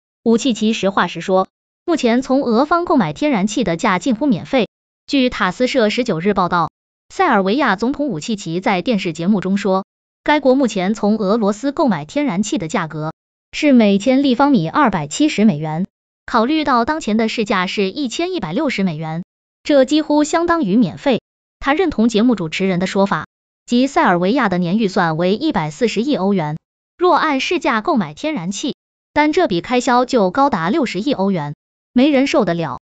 武契奇实话实说：目前从俄方购买天然气的价近乎免费
目前从俄方购买天然气的价近乎免费 据塔斯社19日报道 塞尔维亚总统武契奇在电视节目中说 该国目前从俄罗斯购买天然气的价格是每千立方米270美元 考虑到当前的市价是1160美元 他认同节目主持人的说法 即塞尔维亚的年预算为140亿欧元 若按市价购买天然气 单这笔开销就高达60亿欧元 武契奇实话实说 这几乎相当于免费 没人受得了